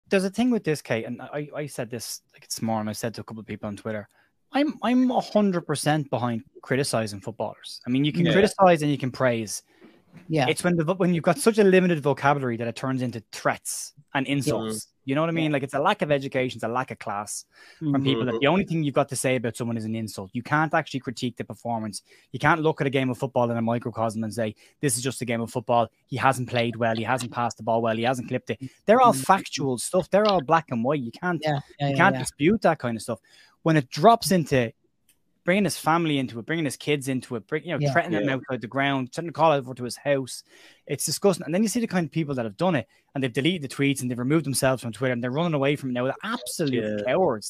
Ger HITS the NAIL ON THE HEAD‼️ ABSOLUTE COWARDS‼️ @LeedsUnitedTheView
Reaction to Patrick Bamford ABUSE

I'm in a Super 6 League. Think you can beat me? and join my league
Code: R544NJ